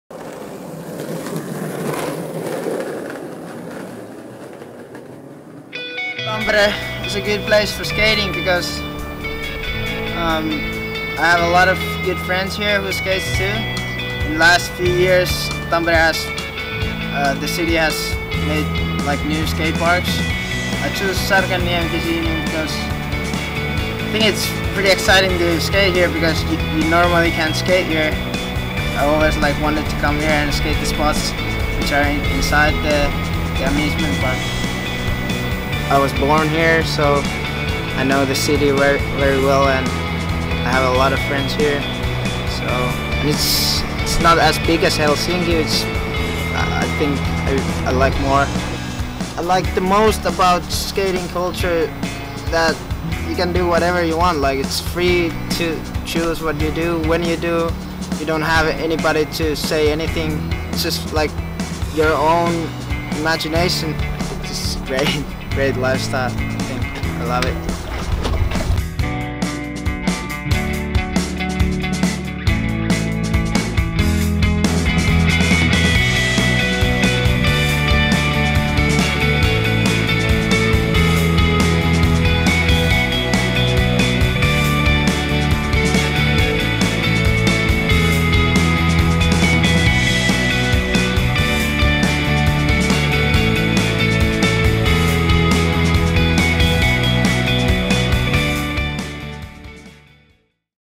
Tampere, Finland - Interview and Making of After Closing Hours" feat. Jaakko Ojanen
Tampere is a great place for skateboarding: in this making of “After Closing Hours” video, the talented Finnish skateboarder Jaakko Ojanen explains why.

Interview and Making of video by Laura Vanzo, Visit Tampere Ltd